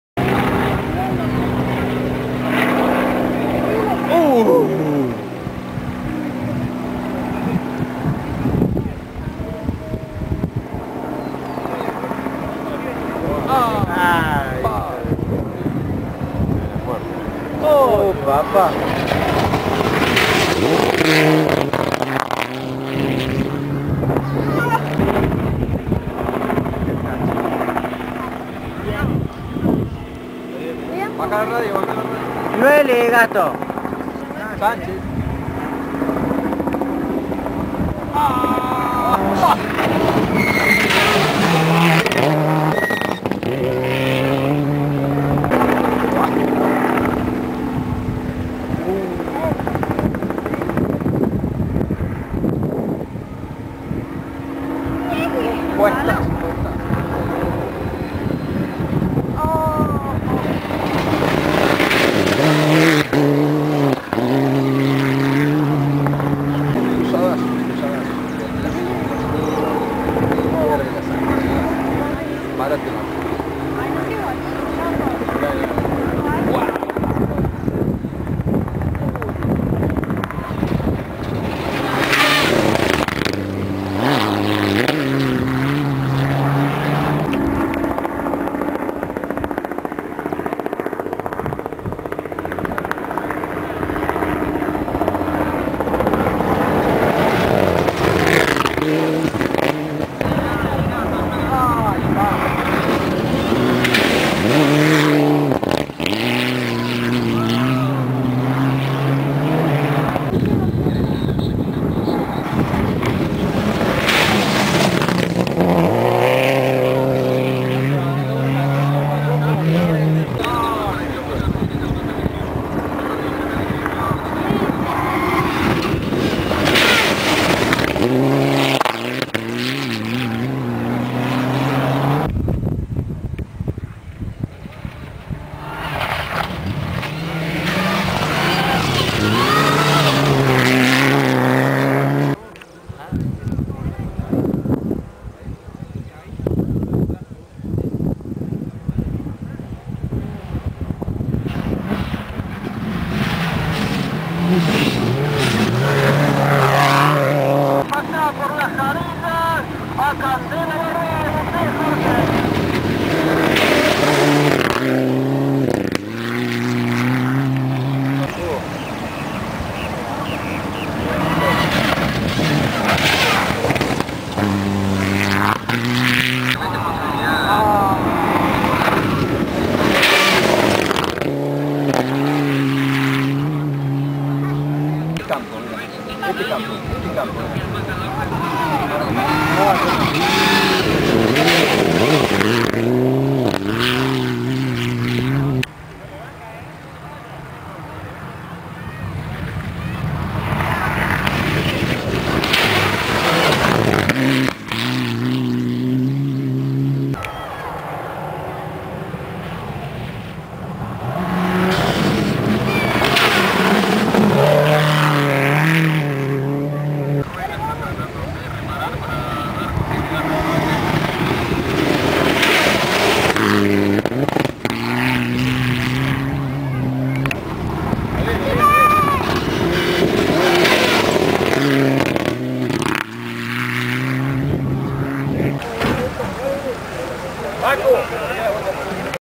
Rally Nacional 07 - Carlos Paz
Primera pasada al tramo las jarillas - Canteras Verdol. Esto es el primer vado a 2000 mts de la largada aproximandamente. Al final del video se ve el auto de Quiroga despues de dar 6 tumbos tras quedarsele el acelerador trabado a fondo. No lo filme al accidente por que se venia hacia a mi el auto y tube que salir corriendo